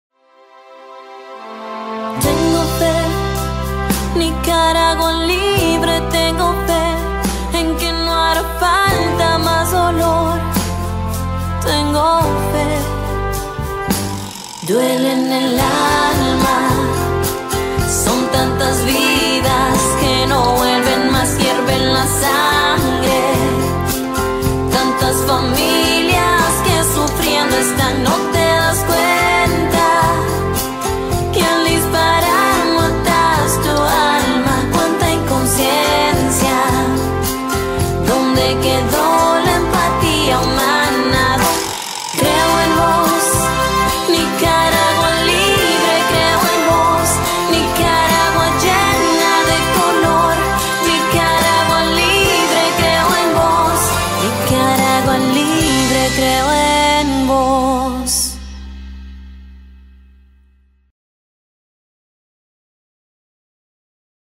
DERECHOS HUMANOS
Radio Dario inicia campaña
Tengo derecho...
Por que los derechos humanos no se negocian.